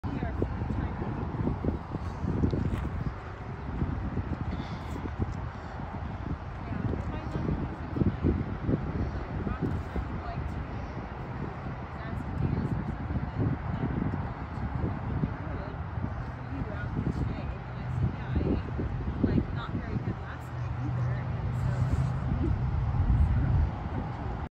1969 VW T2 Westfalia, Walk Around, Ventura CA
Enjoying the day out in Ventura, CA short clip of our 1969 VW Westfalia.  Trying to build up more VW videos @robertsoldcars Thanks for looking